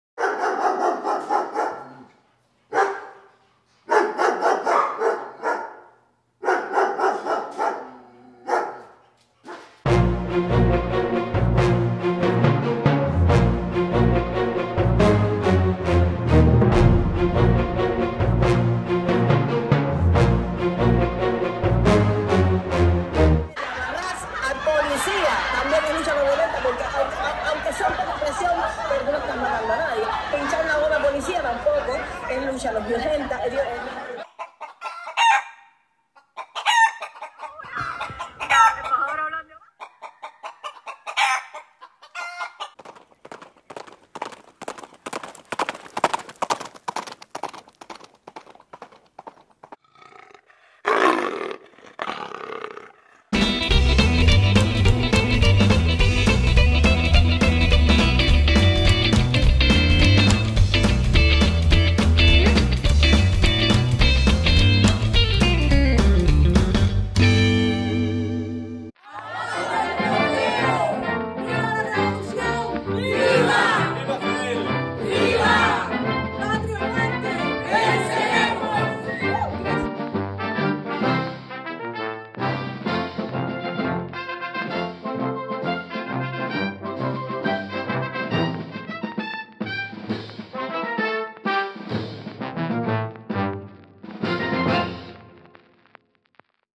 Creatures of darkness in Stockholm HD 1080p
Creatures of darkness in Stockholm, is about forces of human evil that attack the most sacred of human rights, violate the most basic rules of coexistence and cause the peaceful coexistence of society in Sweden.
Sounds taken from i Move and some photos from Pixabay.